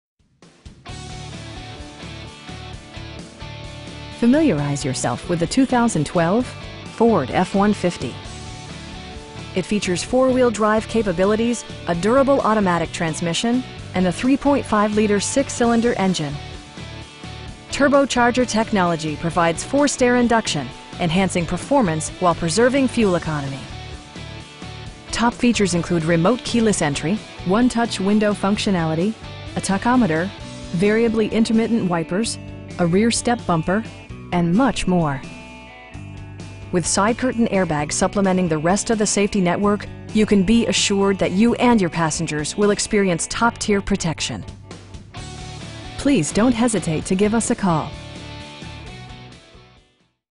2012 Ford F-150 Lariat in Fergus Falls, MN 56537
Nelson Auto Center

Familiarize yourself with the 2012 Ford F-150. It features four-wheel drive capabilities, a durable automatic transmission, and a 3.5 liter 6 cylinder engine. Turbocharger technology provides forced air induction, enhancing performance while preserving fuel economy. Top features include remote keyless entry, 1-touch window functionality, a tachometer, variably intermittent wipers, a rear step bumper, air conditioning, power door mirrors, and much more. With side curtain airbags supplementing the rest of the safety network, you can be assured that you and your passengers will experience top-tier protection. We pride ourselves on providing excellent customer service. Please don't hesitate to give us a call.